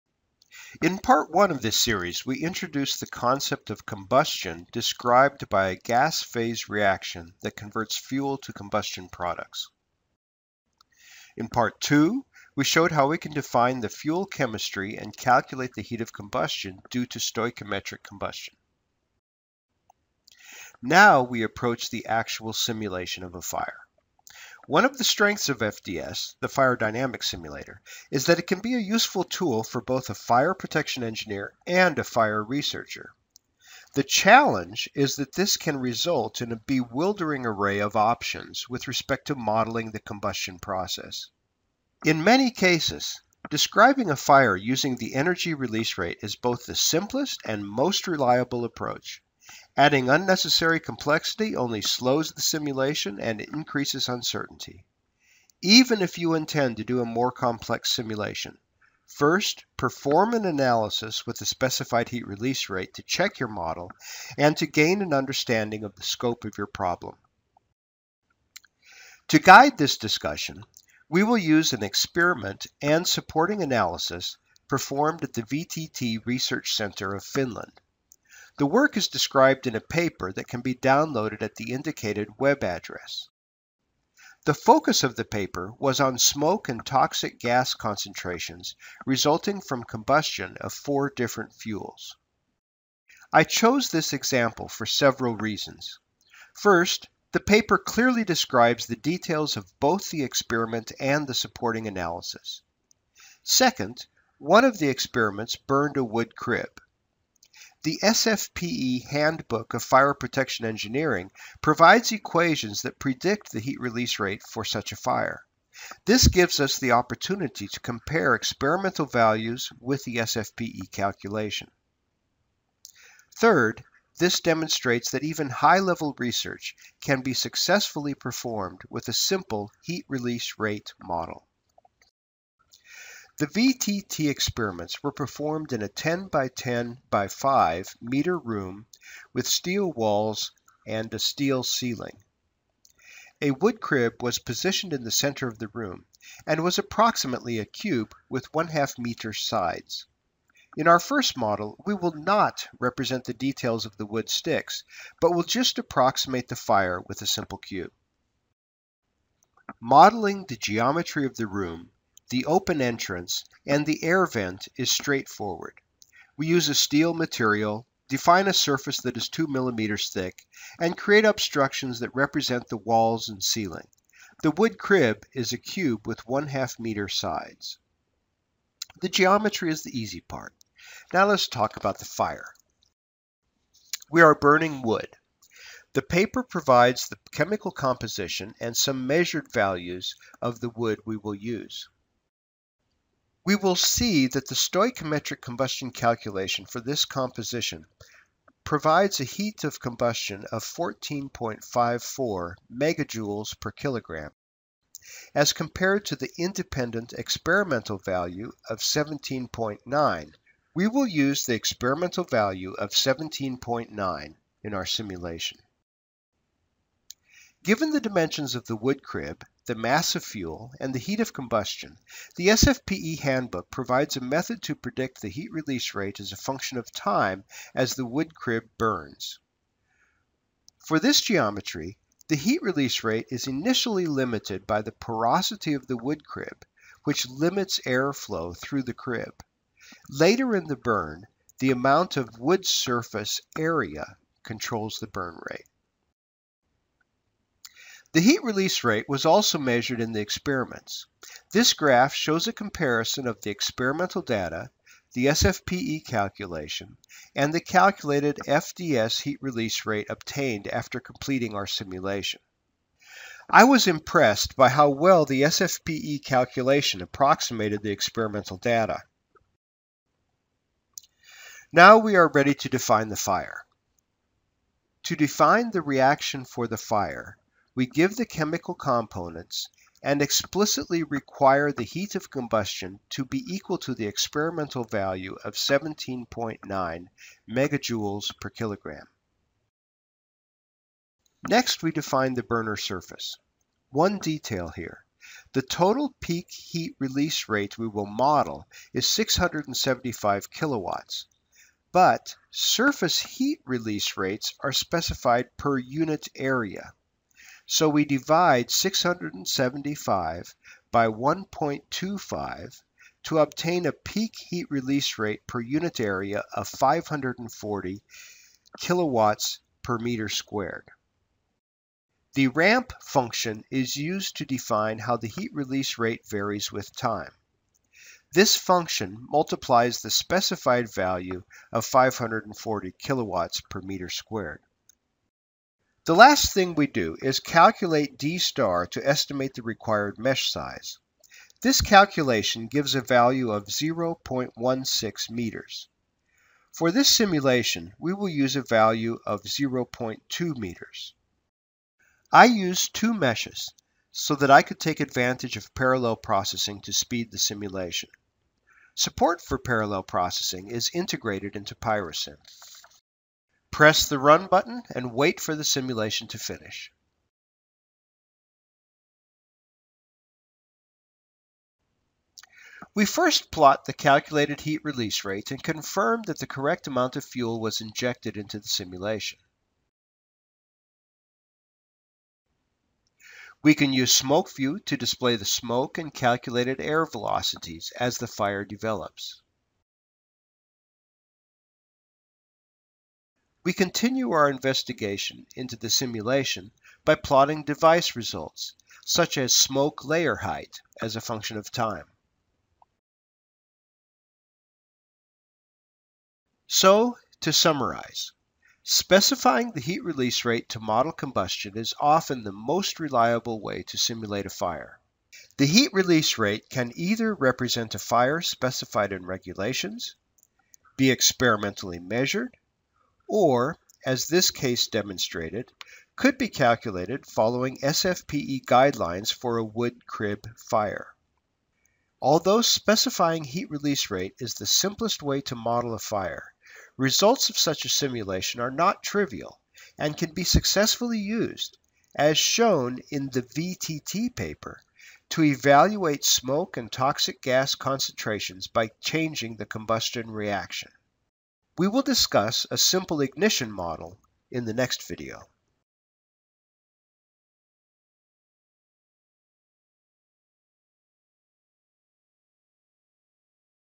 PyroSim: Modeling Fire, Part 3 - Combustion using HRR
In this video we demonstrate how to model a fire using a specified heat release rate. The chosen problem allows us to compare our result with experimental data and the SFPE calculation for a wood crib.